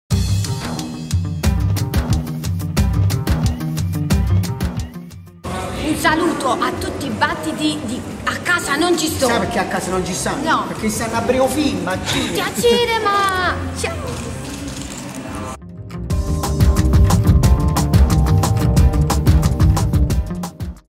Il Saluto di Enzo e Monica degli Arteteca A Casa Non Ci sto
Gli Arteteca salutano tutti gli amici che ci seguono sul nostro Portale Blog.